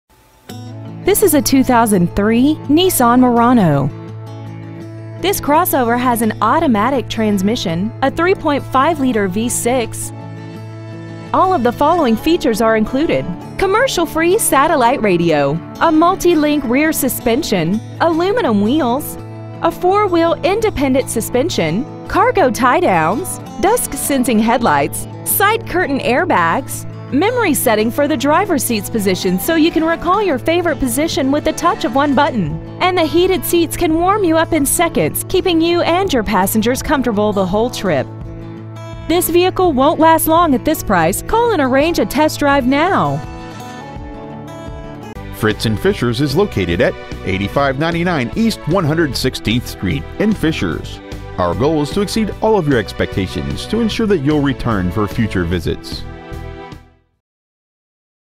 2003 Nissan Murano Noblesville IN
Year : 2003
 Make : Nissan
 Model : Murano
 Engine : 3.5L V6 24V MPFI DOHC
 Trans . : Automatic CVT
 Exterior : Bronze
 Miles : 195,864
 Interior : Charcoal
 Stock : 9992